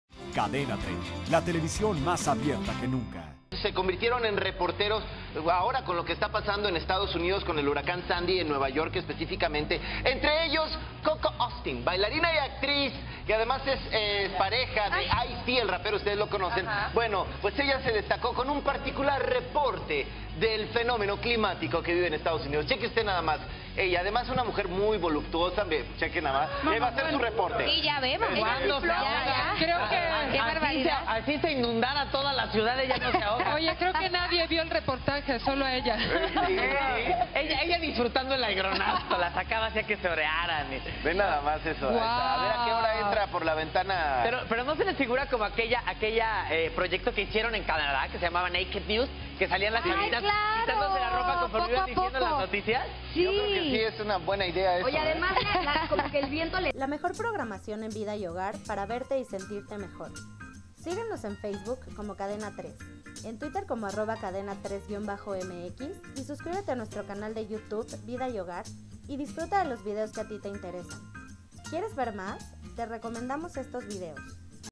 ¡Nuestro Día! Famosos aprovechan el fenómeno meteorológico para salir a cuadro
Muchos famosos se aprovechan de lo ocurrido en Estados Unidos para salir a cuadro